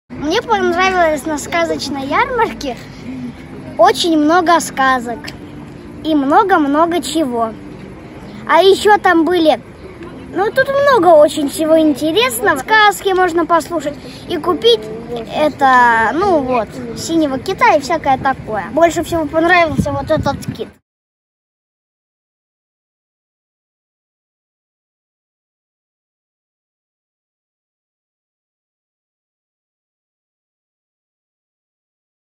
Ярмарка СКАЗОЧНЫЙ МИР, первые впечатления. г. Новосибирск
Впечатления от встречи с ярмаркой СКАЗОЧНЫЙ МИР.
Артём, г. Новосибирск.